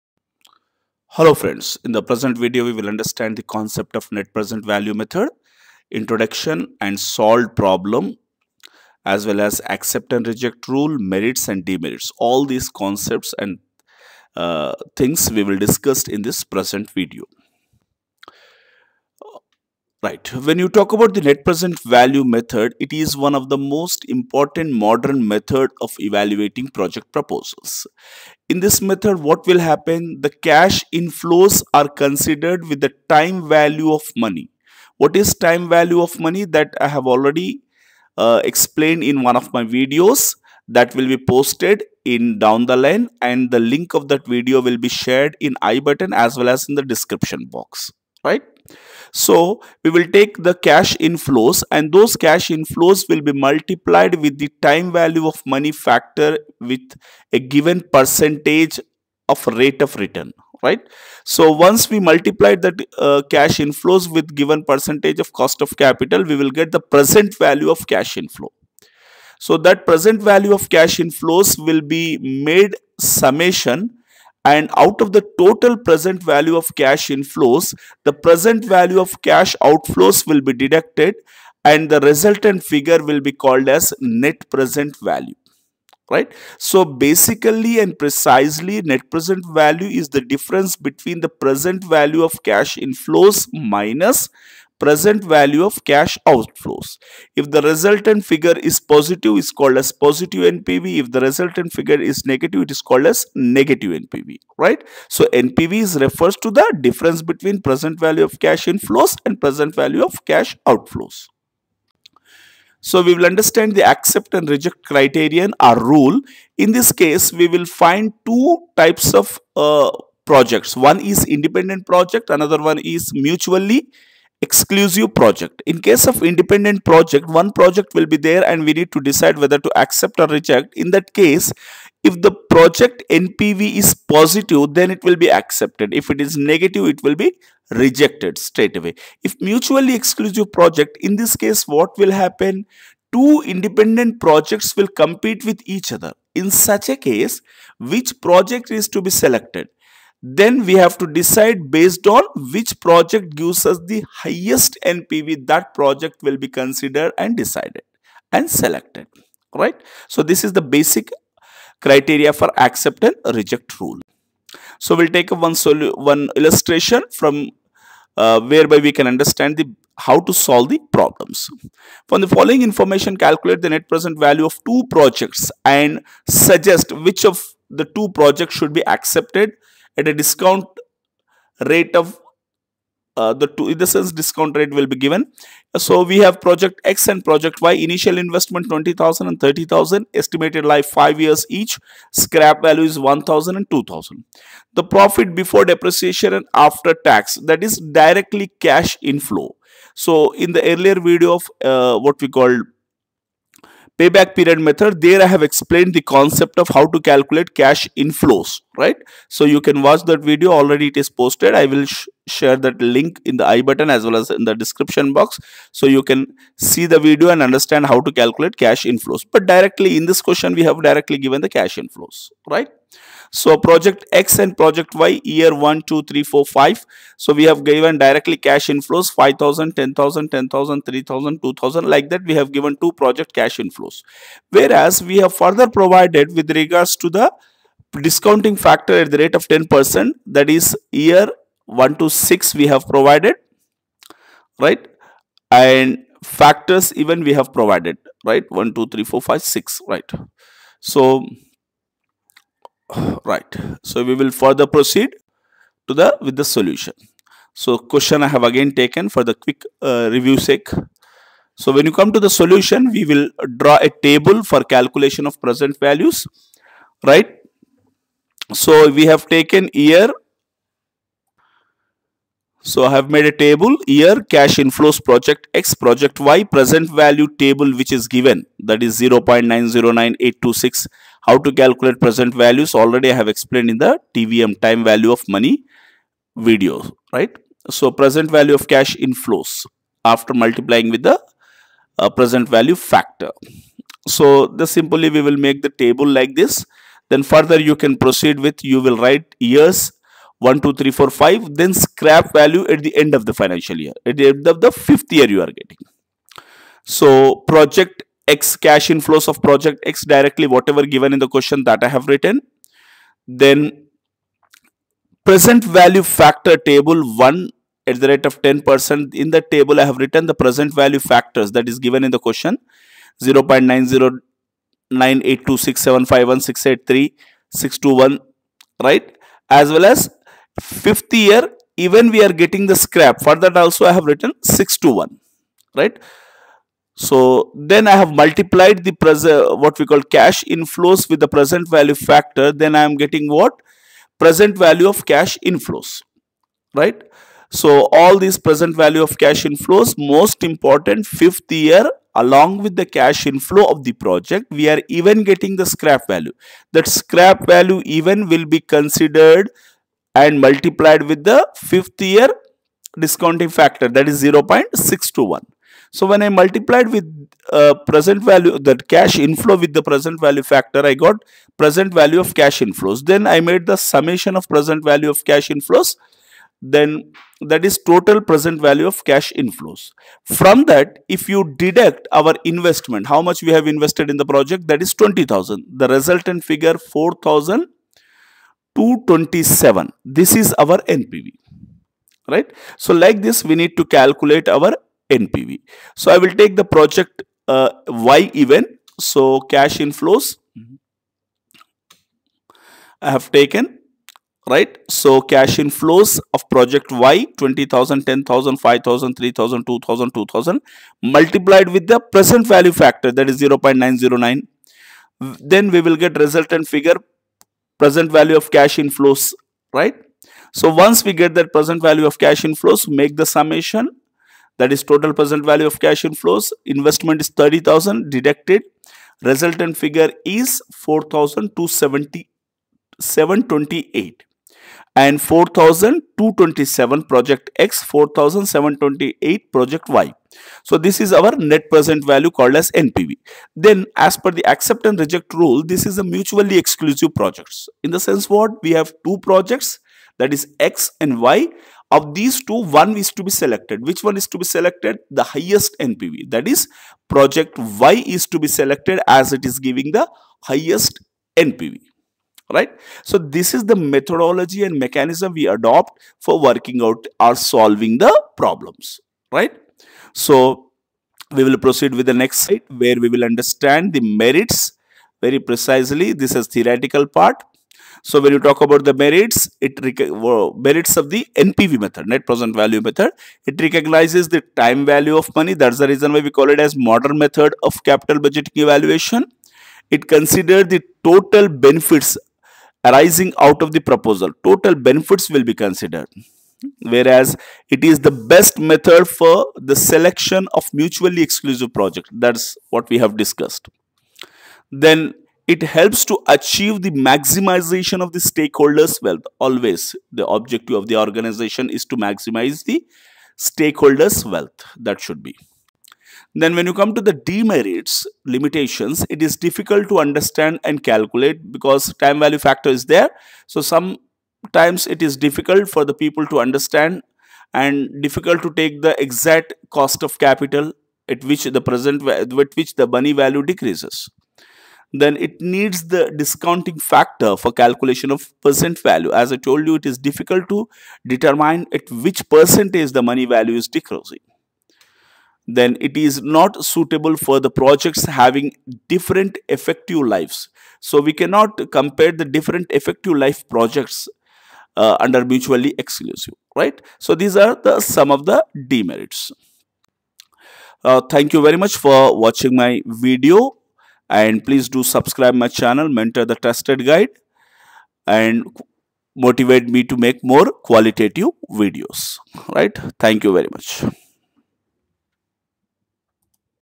Net Present Value || NPV Solved Problem || NPV for B.com, M.com, BBA, MBA, CA, CS & CMA.
In the present video Net Present Value Method Introduction, Accept and Reject Rule, Merits, demerits and Solved Problems is explained in very simple and easy manner to make the students understand the concept.